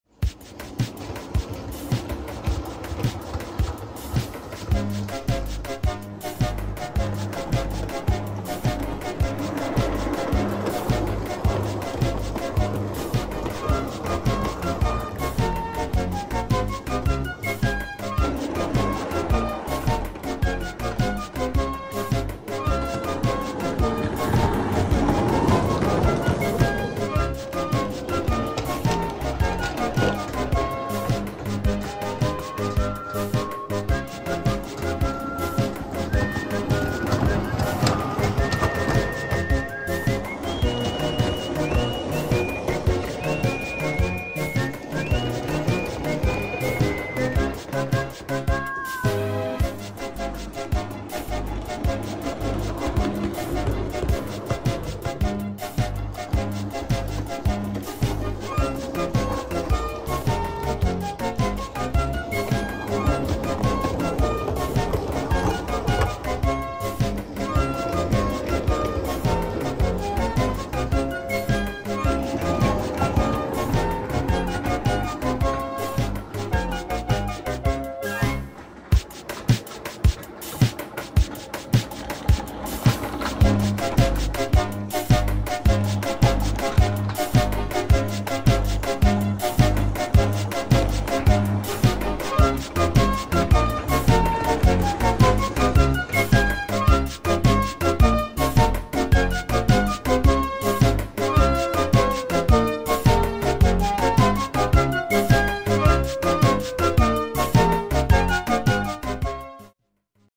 LARGE SCALE TOBY PULLING TRUCKS AND COACHES | THOMAS AND FRIENDS
Another little montage , This time with Toby pulling different trucks and coaches around the layout.